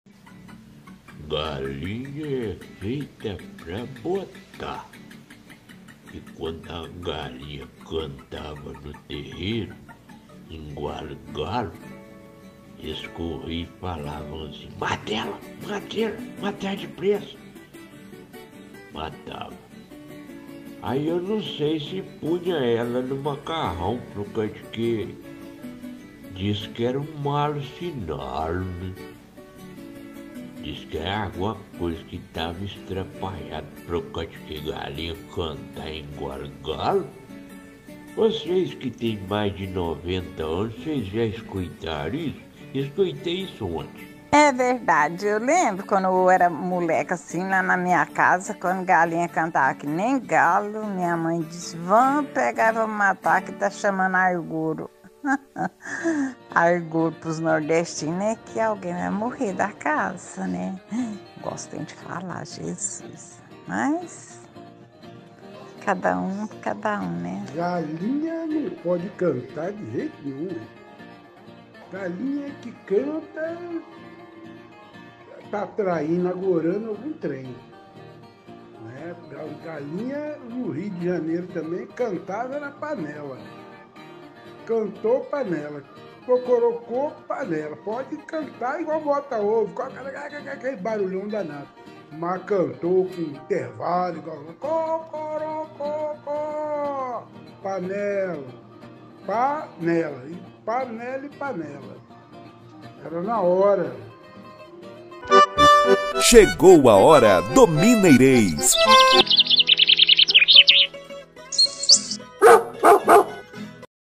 GALINHA NÃO PODE CANTAR IGUAL GALO, OS ANTIGOS FALAVAM
GALINHA NÃO PODE CANTAR IGUAL GALO, OS ANTIGOS FALAVAM. Escutem o porquê...
Compartilhe as prosas com seus amigos e divulgue a cultura caipira e as prosinha miudinhas.